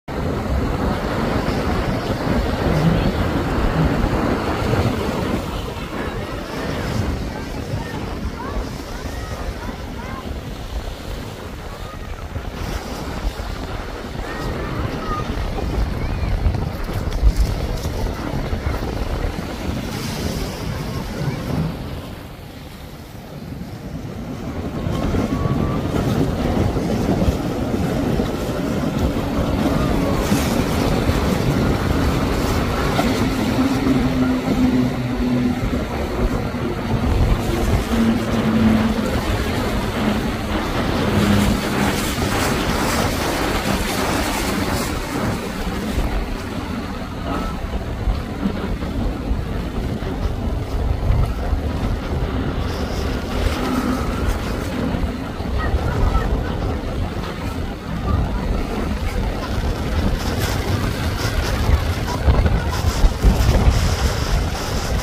Boat Got Engulfed By Big Waves 😱😱
Haulover inlet: This is the most fantastic place in south Florida where 2 beaches meet by a river.